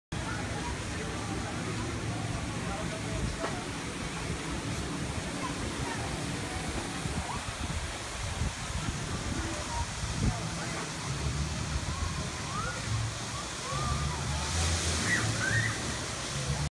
Dreams Playa Mujeres Junior Suite Partial View
Tour of Balcony view of Junior Suite at Dreams Playa Mujeres

More info on Extra Mile Travel